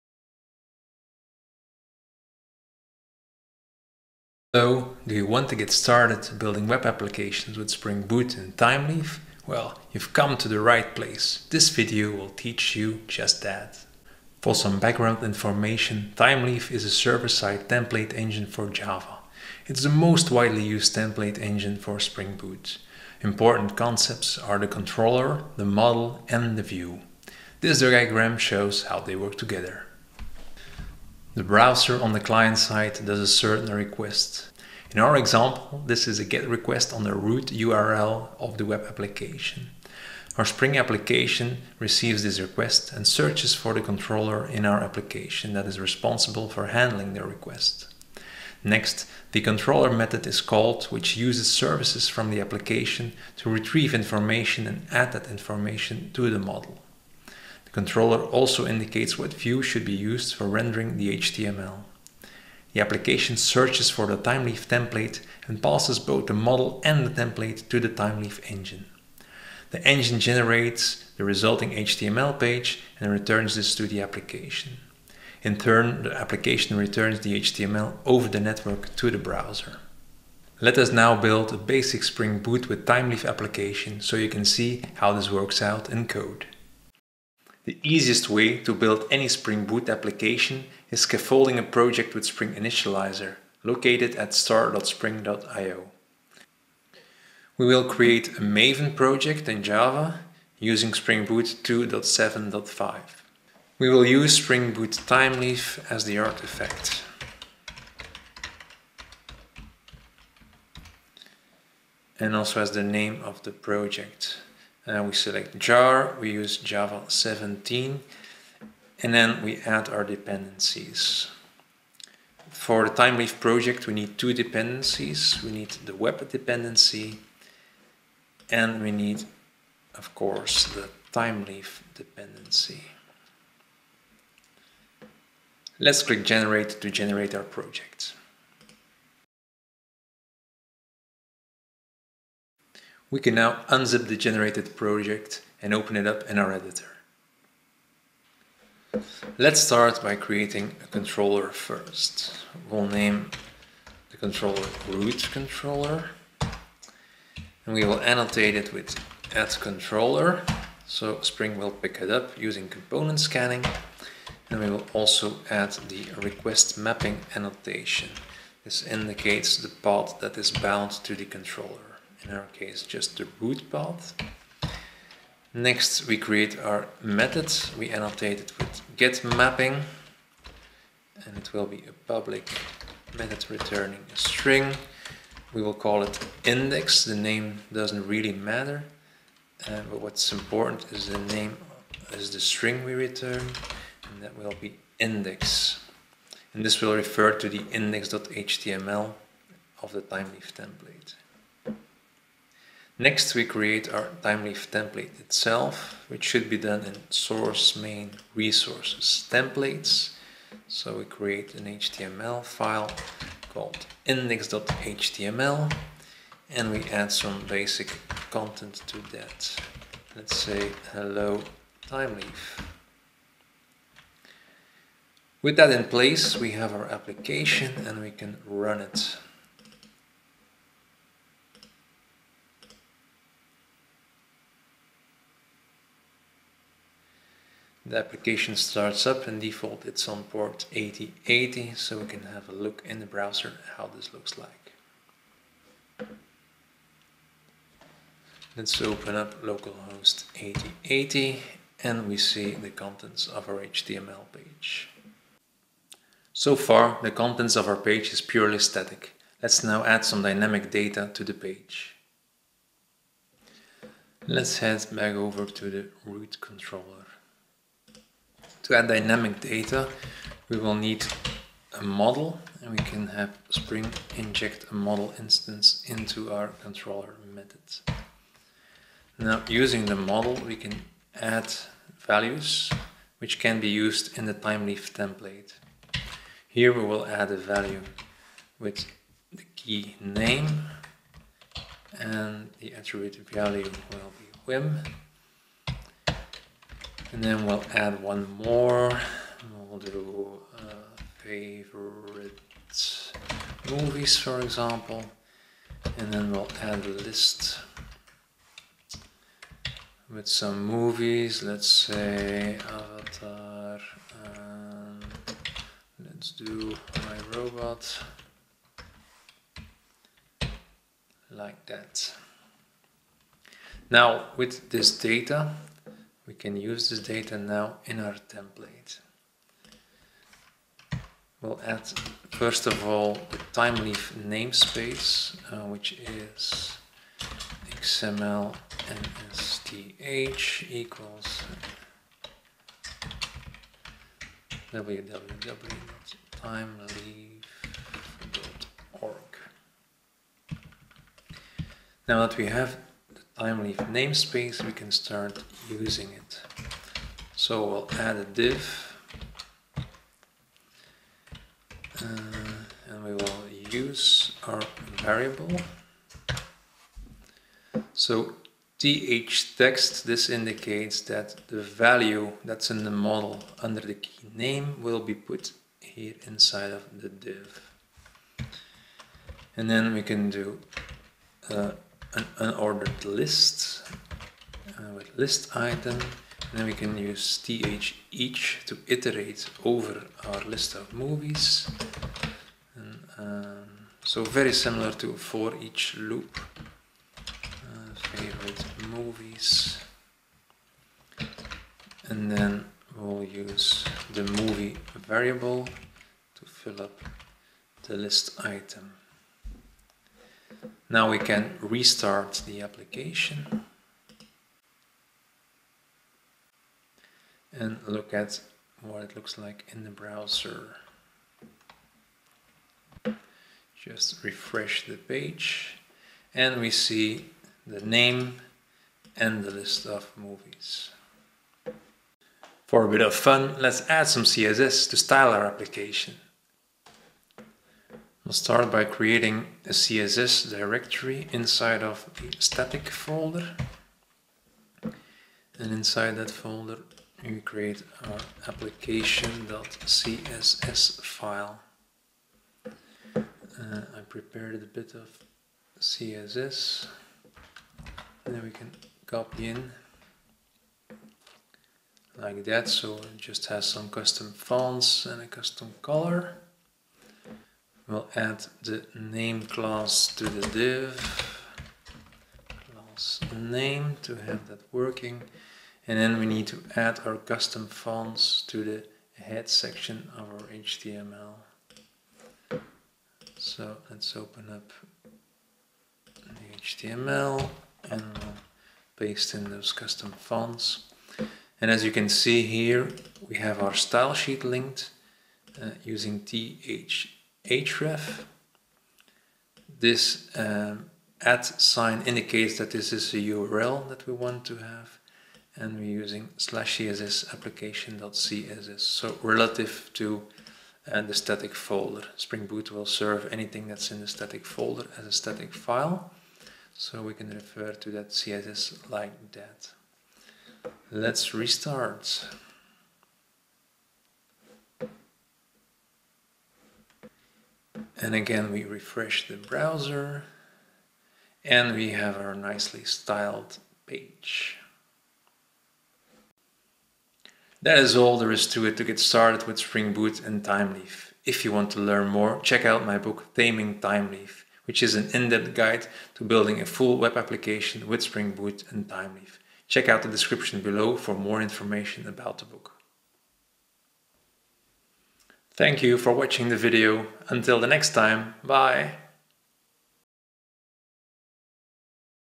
Getting started with Spring Boot and Thymeleaf
This video will show how to get started with Spring Boot and Thymeleaf using Spring Initializr. We will build a simple page with some dynamic data. We will also add some CSS to show how you can style your page.

Chapters:

00:00 Introduction
00:24 How does Thymeleaf work?
01:22 Getting started
04:15 Adding dynamic data
07:12 Add CSS
08:56 Outro